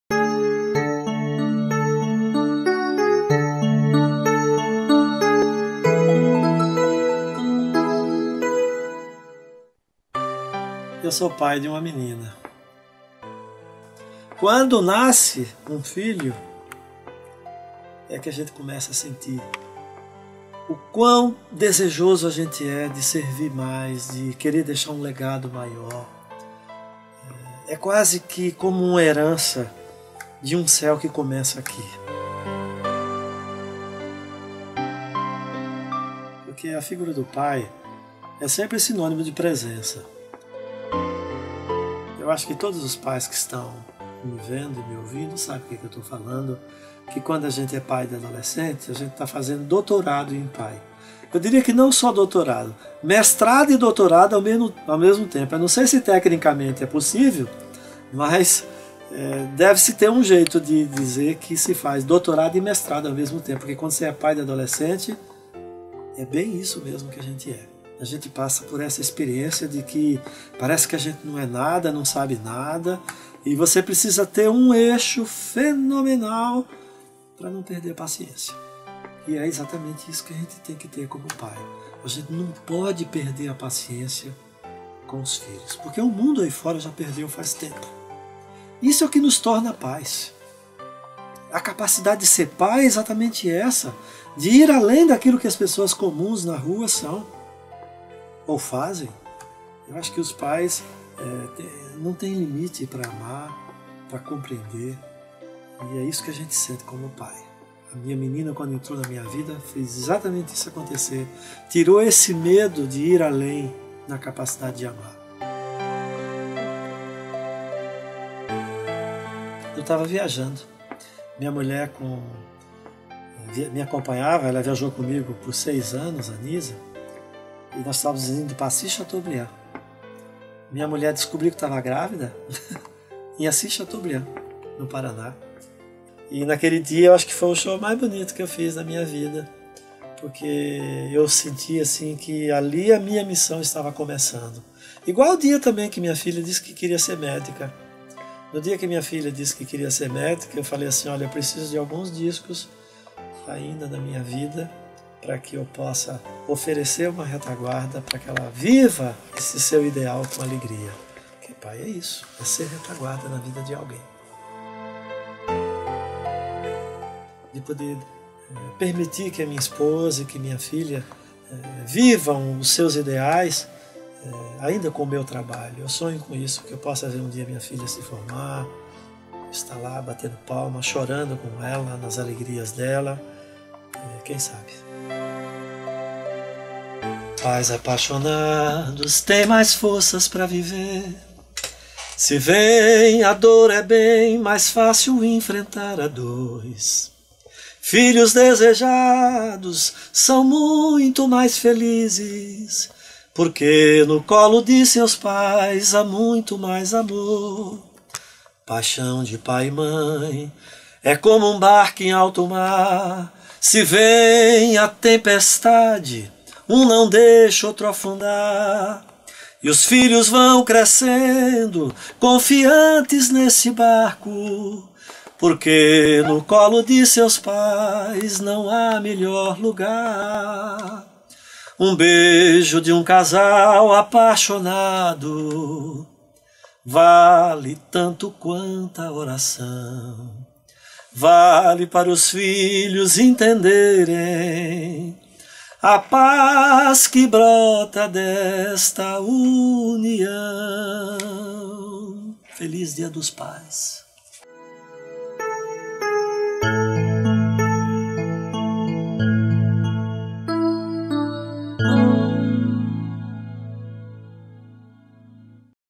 Antonio Cardoso - Histórias de Pai
Lembrando da importância e da missão de ser Pai, o cantor e compositor Antonio Cardoso, partilha sobre a sua experiência de paternidade.

"A capacidade de ser pai é exatamente essa de ir além daquilo que as pessoas comuns na rua são, ou fazem. Acho que Os pais não tem limite para amar, para compreender. E é isso que a gente sente quando é pai. A minha menina quando entrou na minha vida, fez exatamente isso acontecer, tirou esse medo de ir além da capacidade de amar." Antonio Cardoso